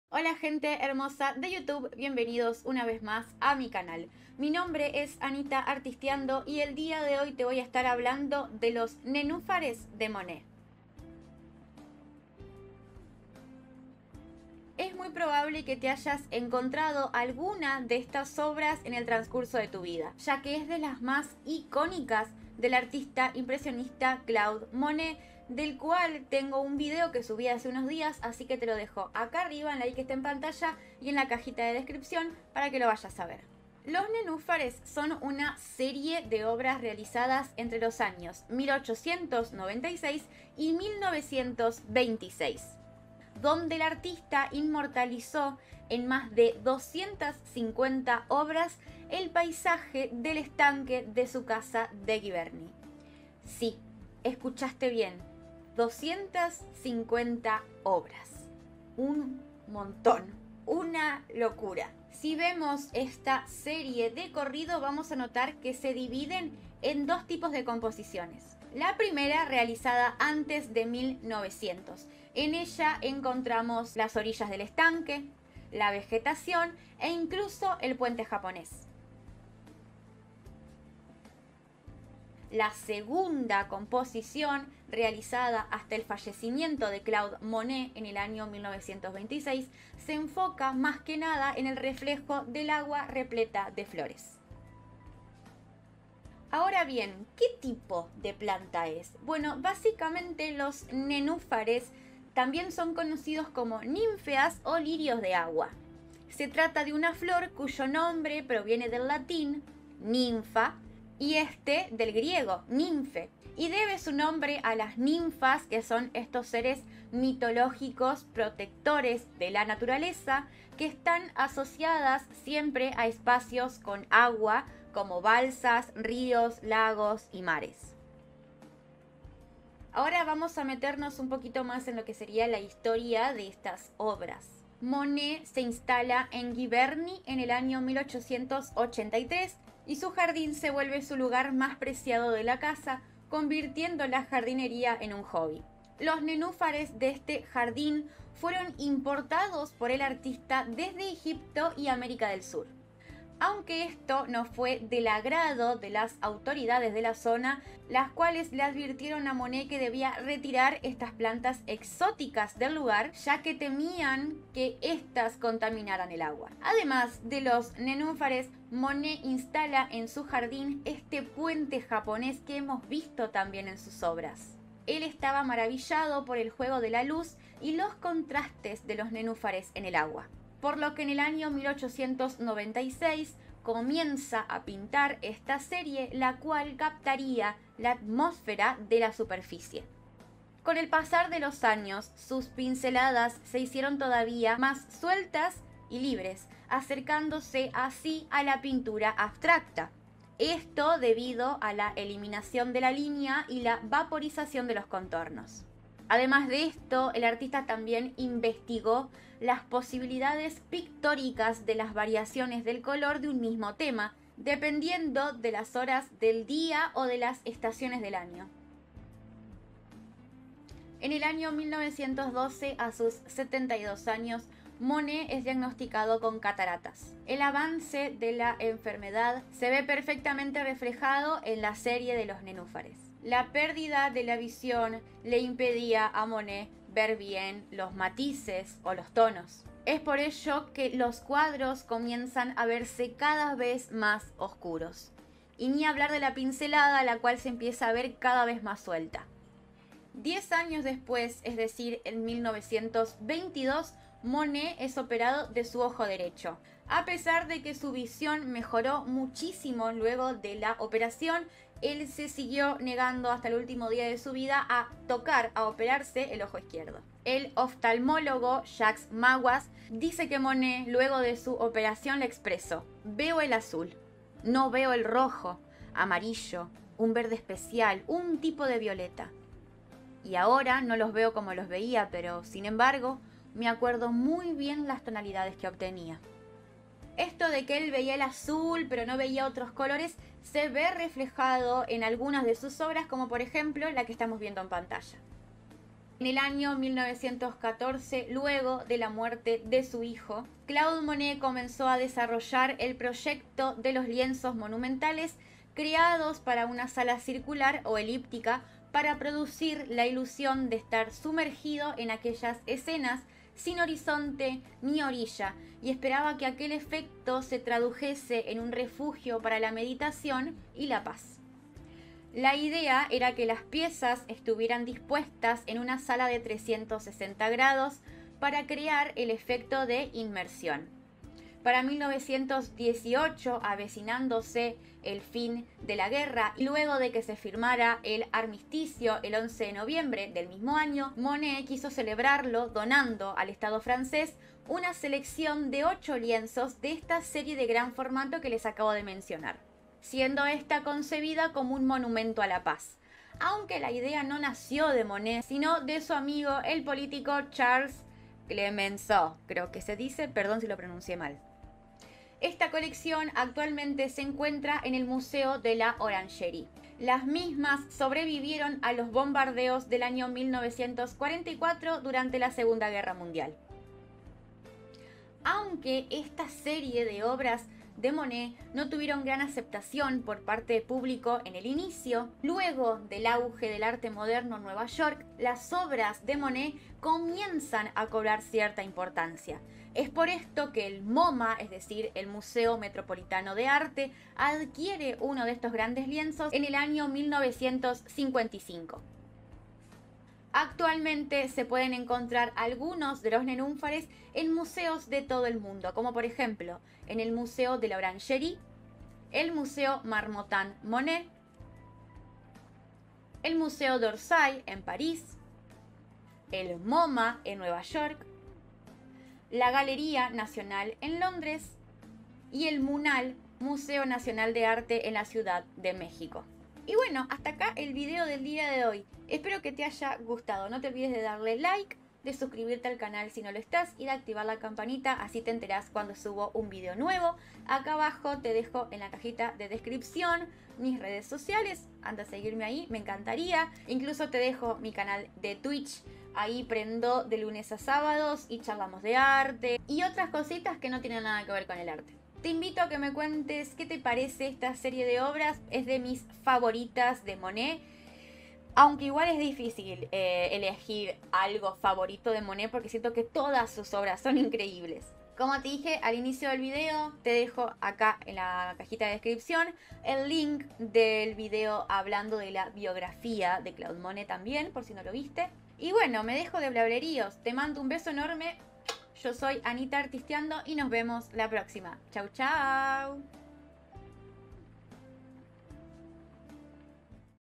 Los Nenúfares de Monet - Artisteando
En  este video les traigo la historia de los "Nenúfares" de Claude Monet.

Redes:
Twitch (prendo de miércoles a sábados de 13 a 16 hs, salvo los jueves que prendo de 13 a 19 hs)